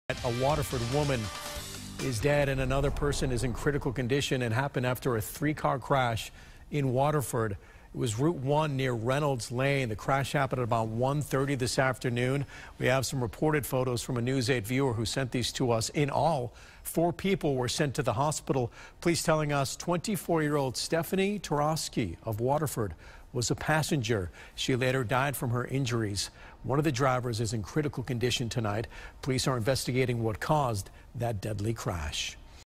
Waterford Police investigate fatal accident on Route 1
Waterford Police say Stephanie Turowski, 24, of Waterford has passed away after a car accident on Monday.